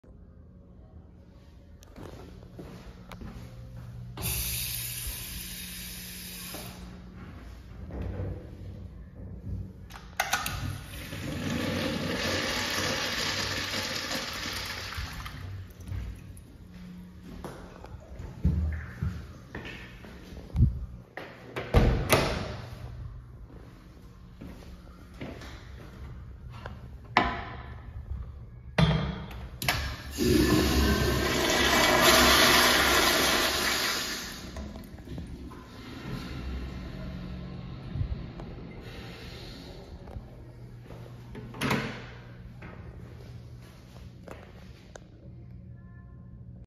681. Marshall’s Men’s Restroom
This restroom is incredibly boring. The Washbrook looks sorta weird.

Fixtures:
1 late 2000’s AS Afwall
1 late 2000’s AS Washbrook

Location:
Swansea, MA